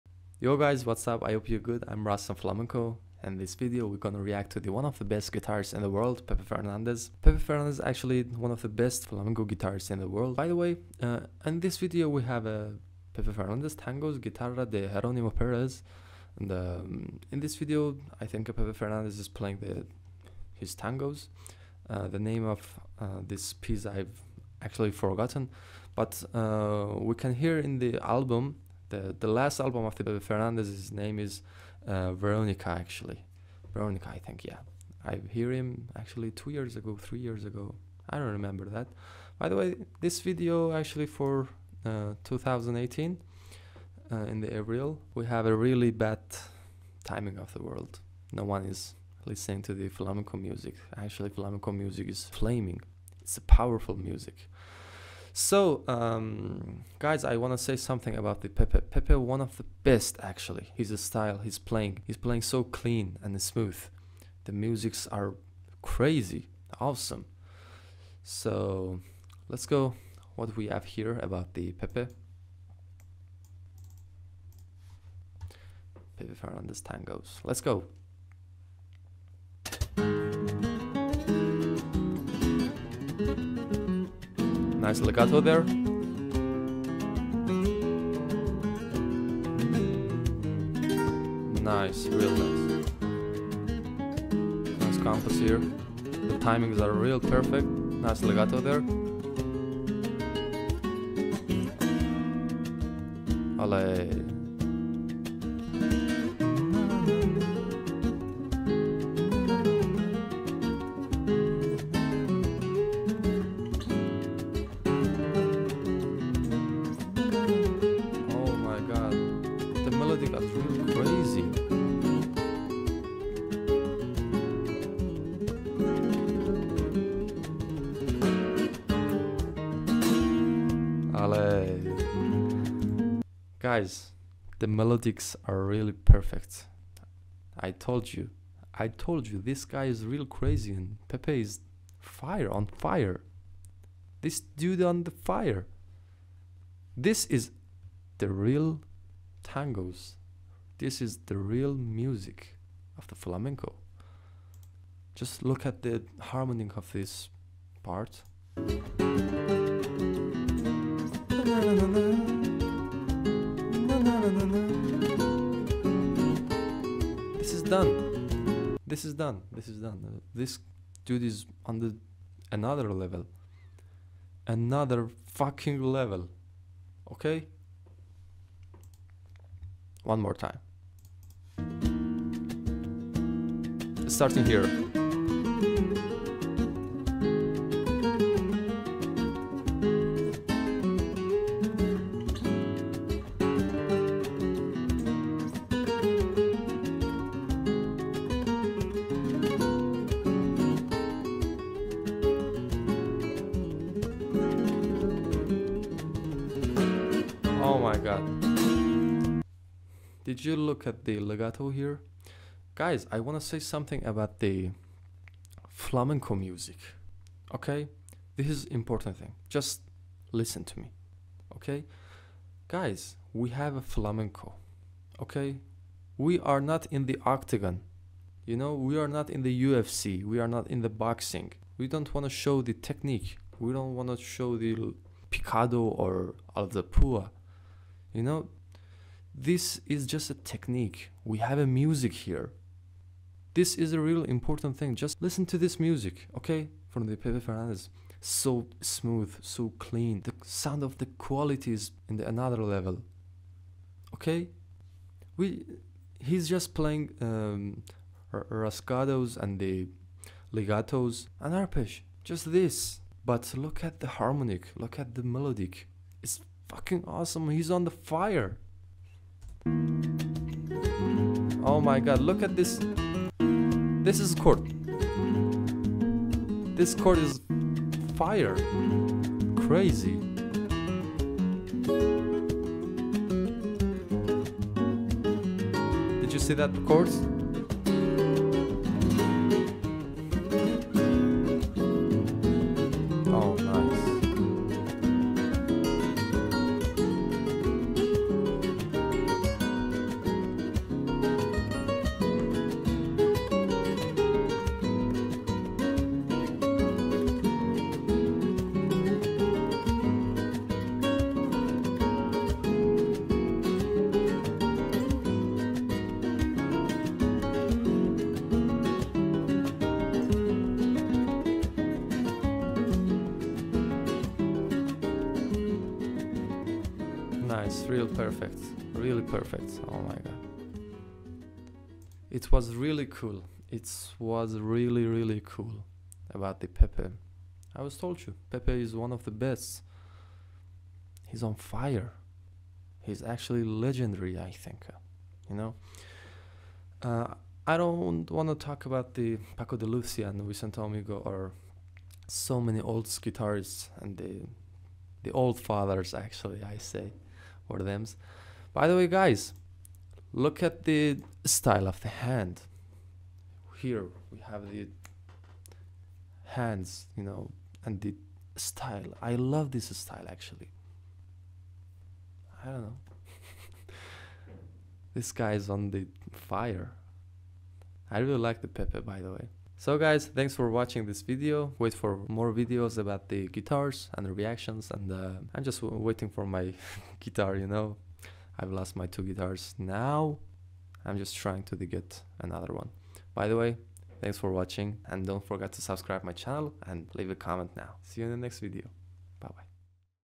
Flamenco Guitarist Reaction To Pepe Fernandez
i reated to one of the best flamenco guitarist in the world .
my friends if u like this video tell me i make more of them :D